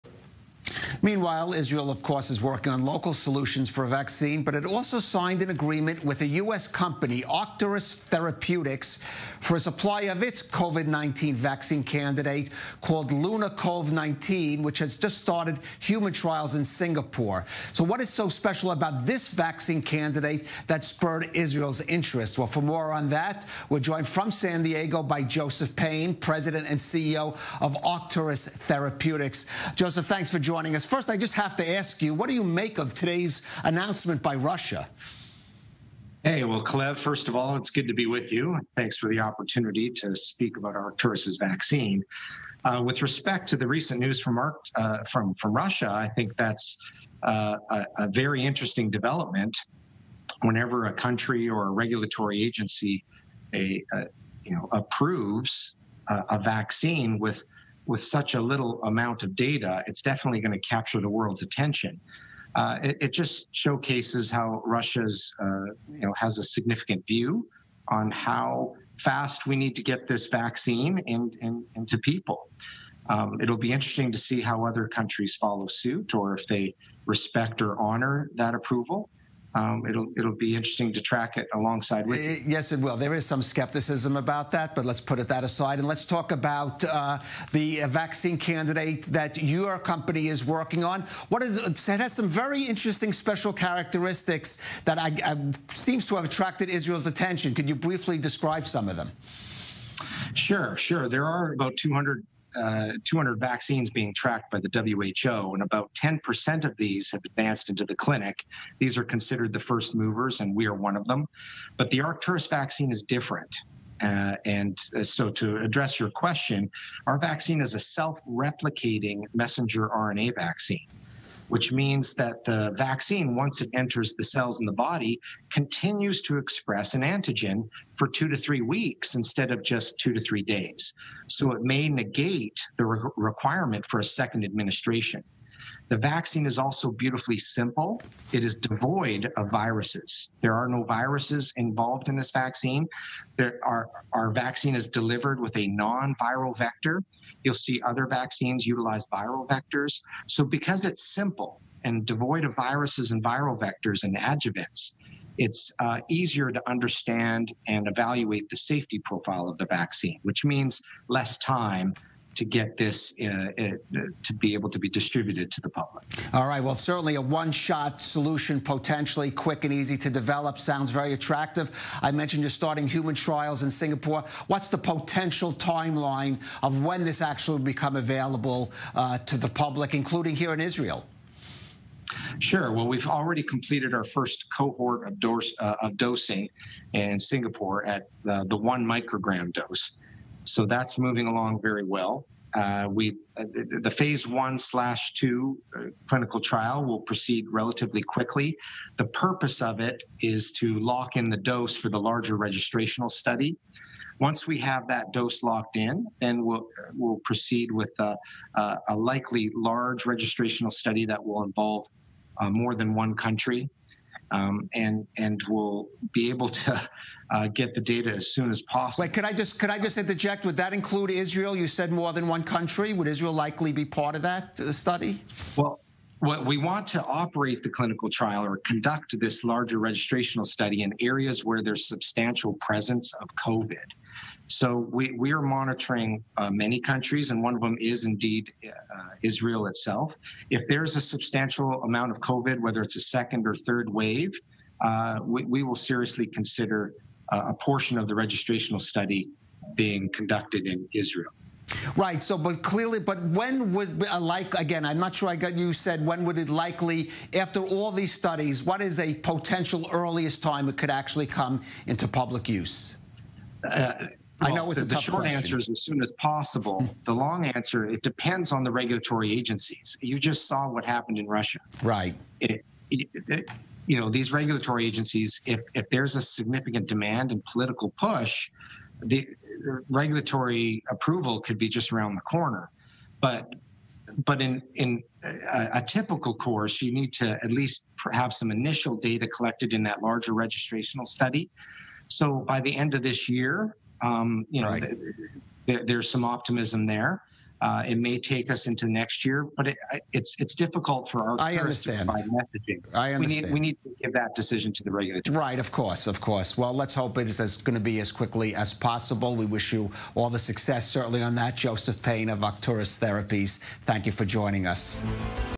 Arcturus Therapeutics' President & CEO, Joseph Payne, Discusses COVID-19 Vaccine Candidate i24 News
Joseph Payne, President and CEO, of Arcturus Therapeutics describes COVID-19 Vaccine candidate on i24 News highlighting vaccine attributes using self-replicating mRNA, vaccine class of being devoid of viruses and viral vectors, and clinical trial status.